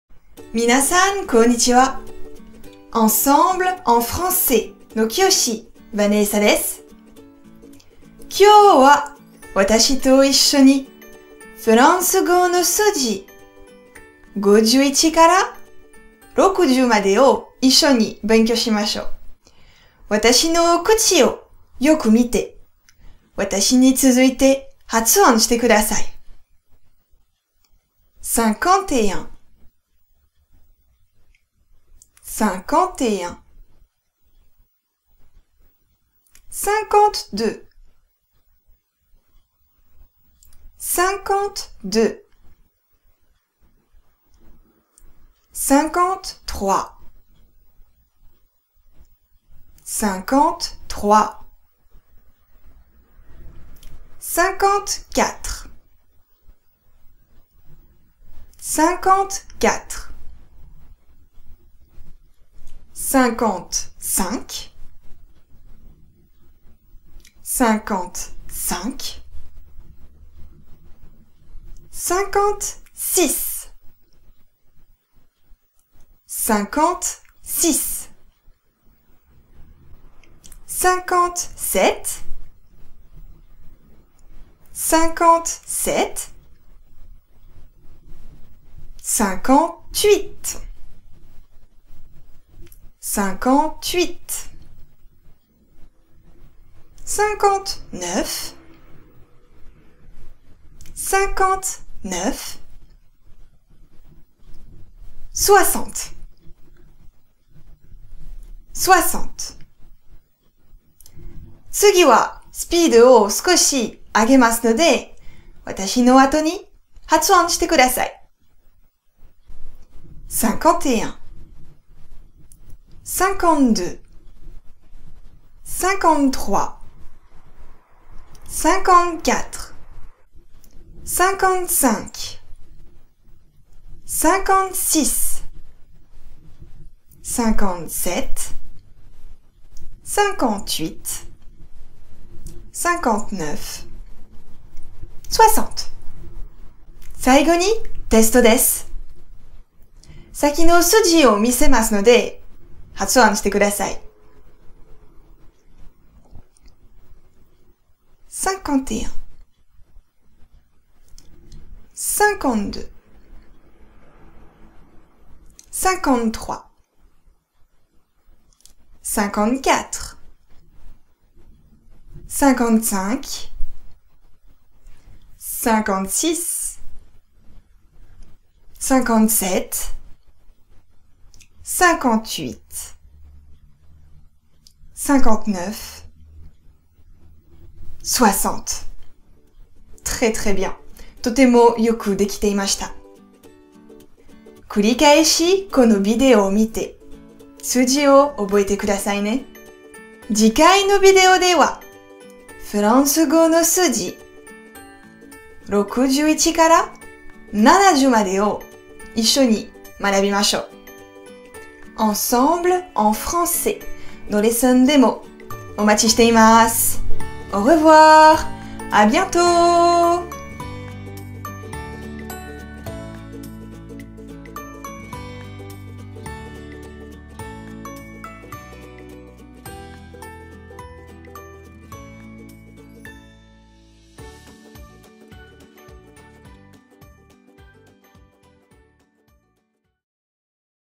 フランス語の数字 発音 51～60【楽しくフランス語】
アンサンブル講師のVanessa先生と一緒に、フランス語の数字「51～60」までを学びましょう。講師の口の開け方や動きをしっかり確認し、効果的にフランス語の数字をマスターしよう！

★定額通い放題！グループレッスン★

★アンサンブルの再生リスト★

★フランス・フランス語情報満載★